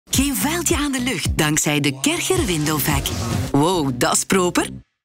De Kärcher Window Vac: voor schitterende ramen!
Reinig makkelijk en zonder strepen ramen en spiegels met de Kärcher Window Vac!